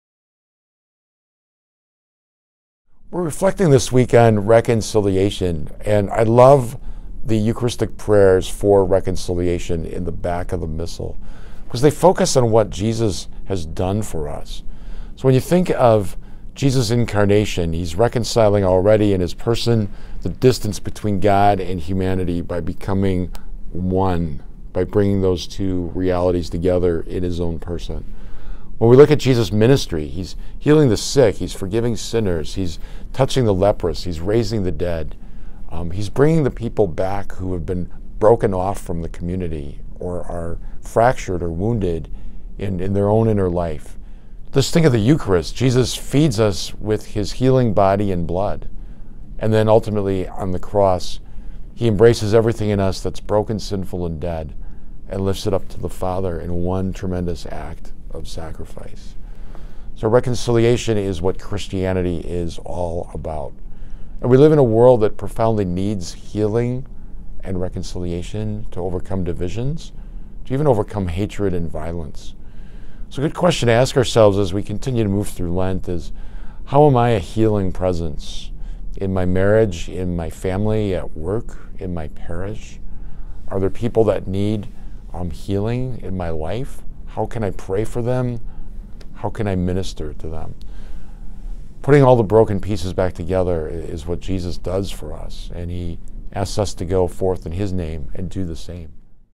March 28, 2019
Bishop Hying reflects on reconciliation.